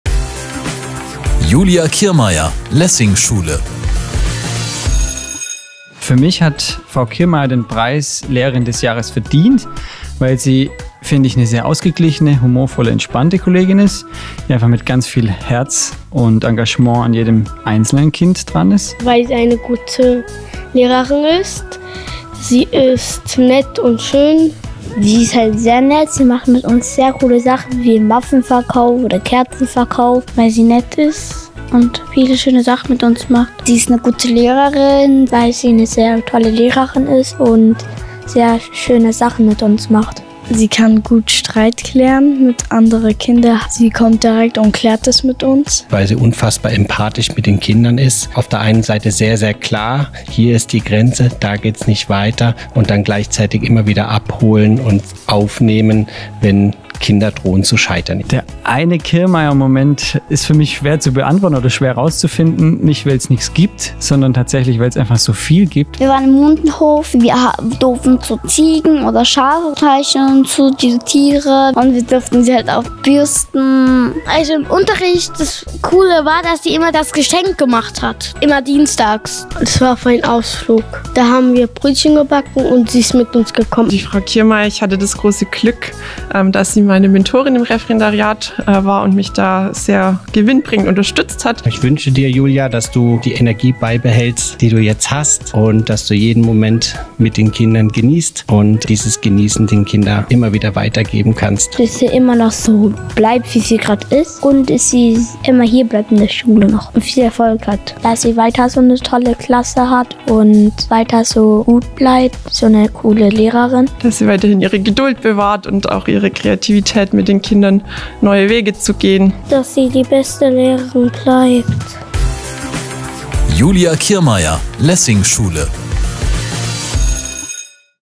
Freiburger Lehrerpreis 2024: Julia Kirmaier, Finalistin
Schülerinnen, Schüler, Kolleginnen und Kollegen der Lessing-Schule (SBBZ) in Freiburg erzählen, warum sie Julia Kirmaier als »Freiburgs Lehrerin des Jahres 2024« nominiert haben.